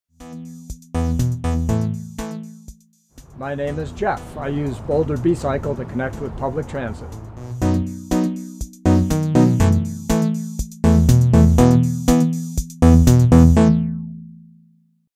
Jeff B-cycles to public transit
Jeff uses Boulder B-cycle, Boulder's nonprofit bike share system, to connect to public transit.